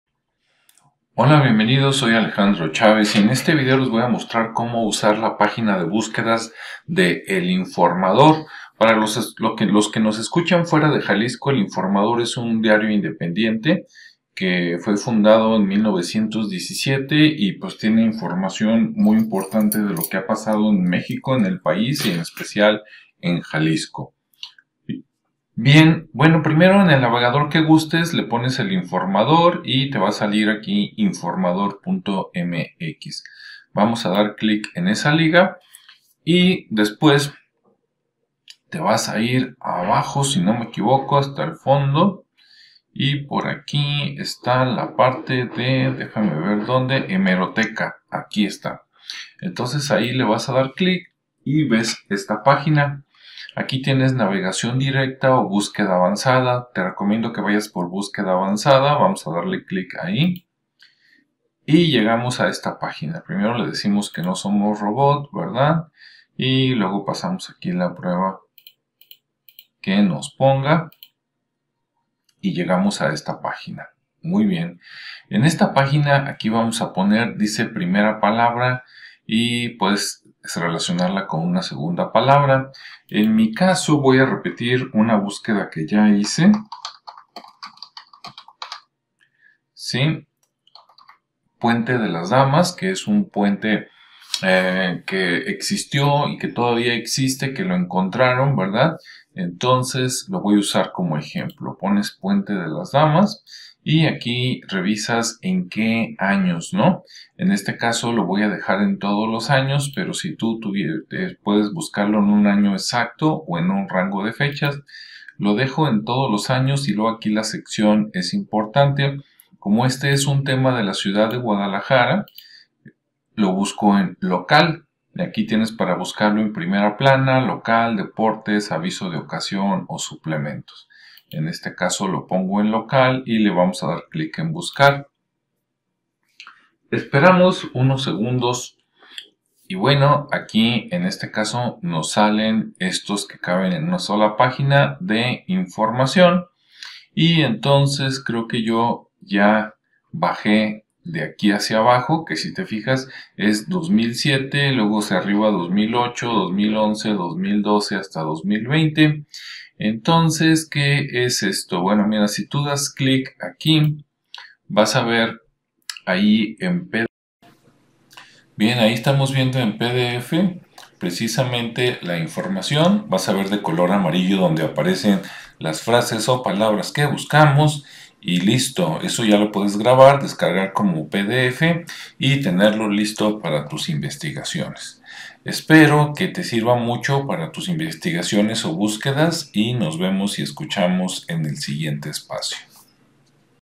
Cómo hacer búsquedas en la hemeroteca del diario el informador
Aprende como puedes hacer búsquedas en la hemeroteca del diario el informador desde tu computadora o celular.